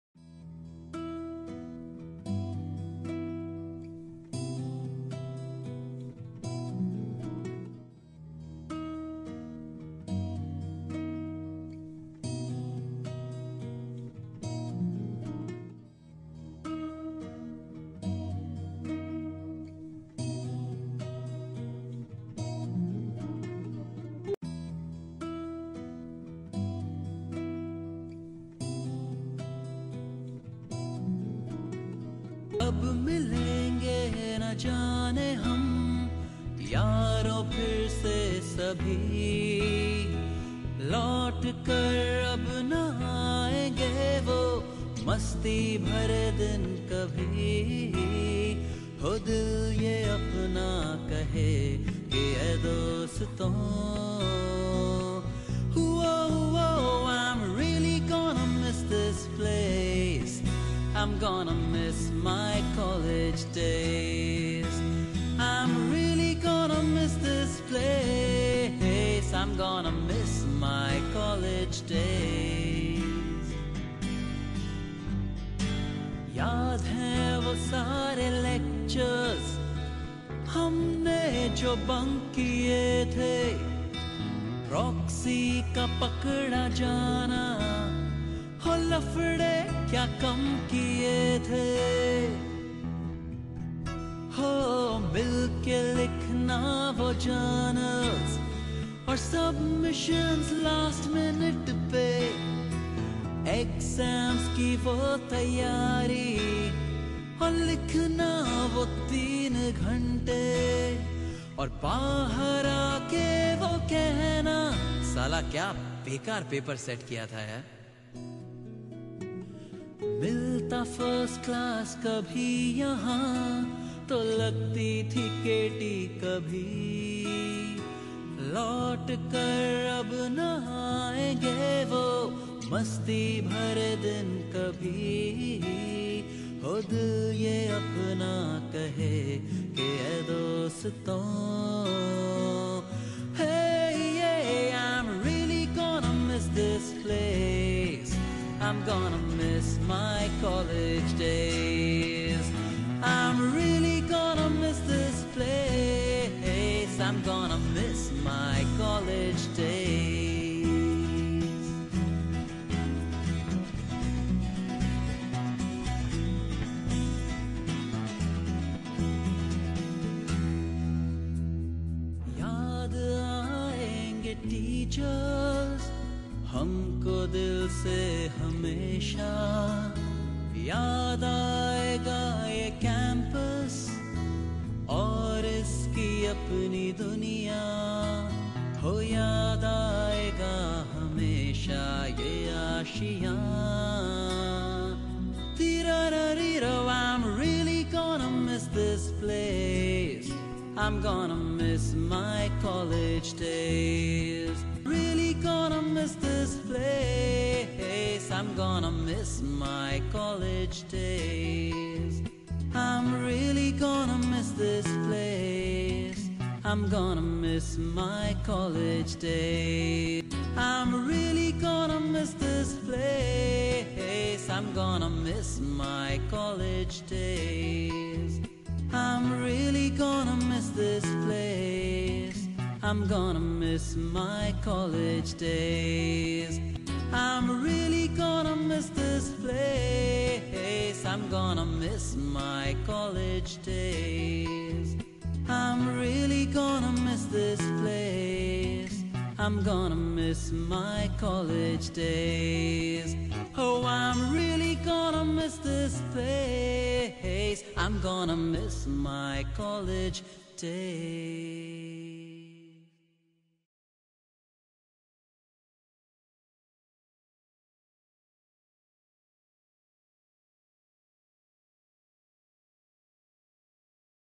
LIFE IN IITTM
This video is made on the theme of LIFE IN IITTM, which reflect the environment and memories of the student studying in this college.
Singer - Gaurav
Team - C.CREW
Spacial Thanks - IITTM Faculties and Our Batch mate (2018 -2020)